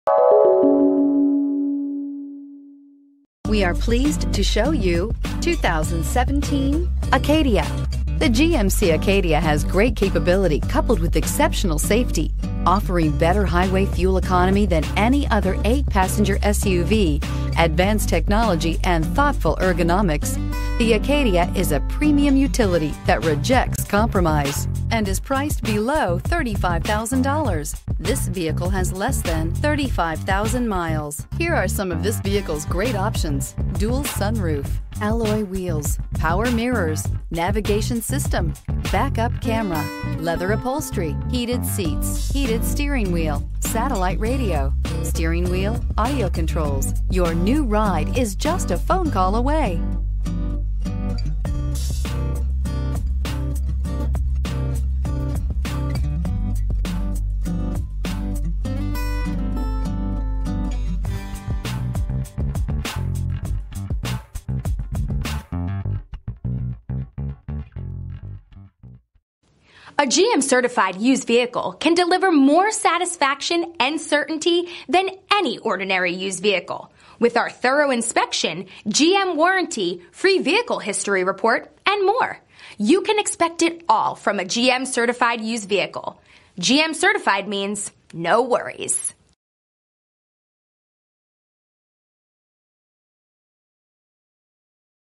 2017 GMC Acadia St. Peters MO P5405
2017 GMC Acadia Denali

This 2017 GMC Acadia Denali is a real winner with features like a push button start, remote starter, backup camera, a navigation system, a leather interior, heated seats, Sirius satellite radio, OnStar Communication System, side air bag system, and digital display.  It has a 6 Cylinder engine.  This one's on the market for $32,888.  It also includes Roadside Assistance, Courtesy Transportation.  This vehicle also comes with 48,000 warranty miles and has been subjected to a rigorous, 172 -point inspection for condition and appearance.  Looking to buy a safer SUV? Look no further! This one passed the crash test with 4 out of 5 stars.  The exterior is a charming ebony twilight metallic.  View this beauty and our entire inventory today! Call for more details.